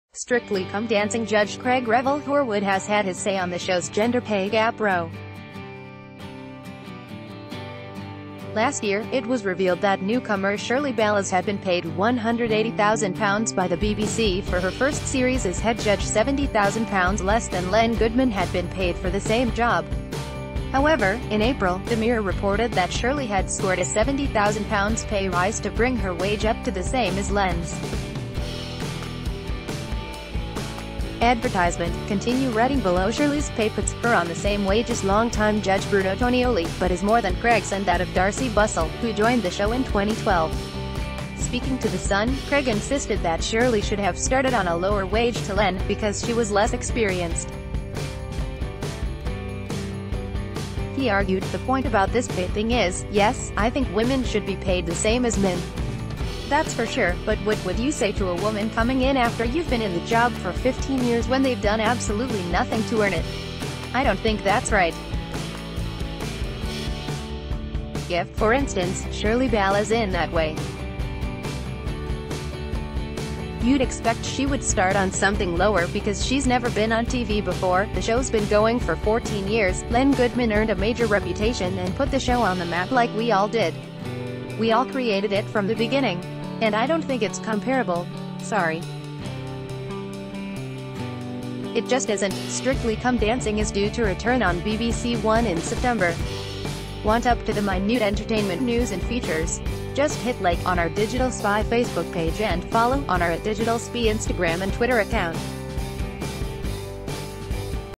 Strictly Come Dancing's Craig Revel Horwood weighs in on pay gap between Shirley Ballas and Len G...
Strictly Come Dancing's Craig Revel Horwood weighs in on pay gap between Shirley Ballas and Len Goodman